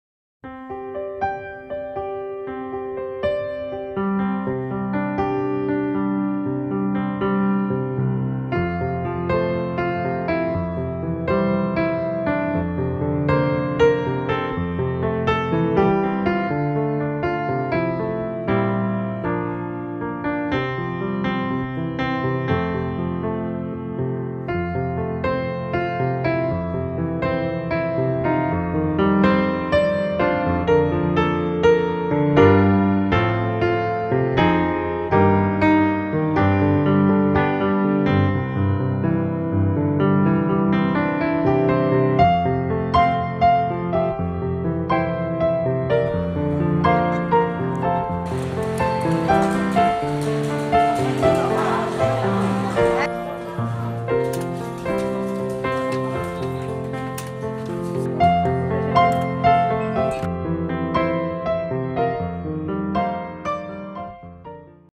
Man gives masks to railway staff for free in C China
Touching moment! A young man hands out face masks for free to staff at a railway station in Changsha, central China's Hunan Province